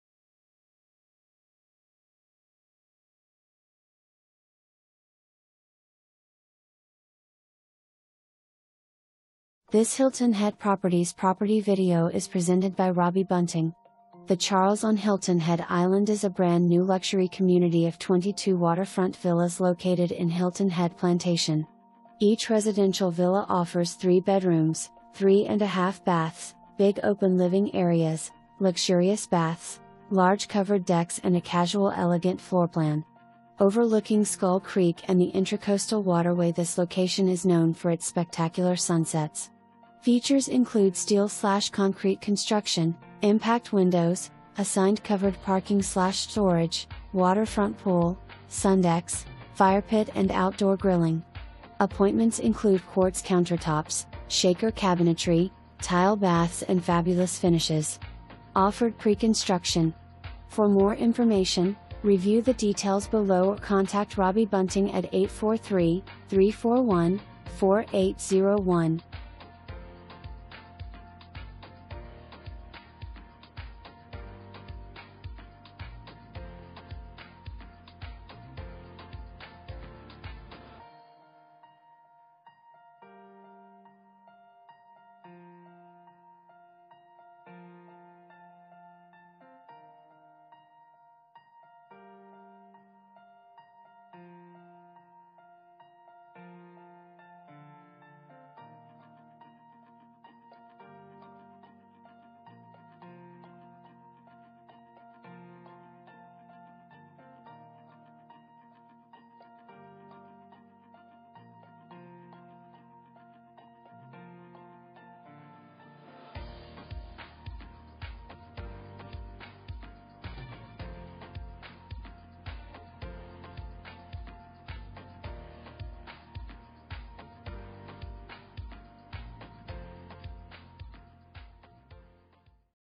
Homes for Sale - 63 Skull Creek Drive, Hilton Head Island, SC
Please enjoy this property video created by Hilton Head Properties

63 Skull Creek Drive
Hilton Head Island, SC 29928
$1,249,900, 3 bed, 4.00 bath, MLS# 414997

The Charles on Hilton Head Island is a brand new luxury community of 22 waterfront villas located in Hilton Head Plantation. Each residential villa offers 3 bedrooms, 3 1/2 baths, big open living areas, luxurious baths, large covered decks and a casual elegant floorplan. Overlooking Skull Creek and the Intra-Coastal waterway this location is known for its spectacular sunsets. Features include steel/concrete construction, impact windows, assigned covered parking/storage, waterfront pool, sundecks, firepit & outdoor grilling. Appointments include quartz countertops, shaker cabinetry, tile baths & fabulous finishes. Offered Pre-Construction.

4 full bath

Robbie Bunting
Hilton Head Properties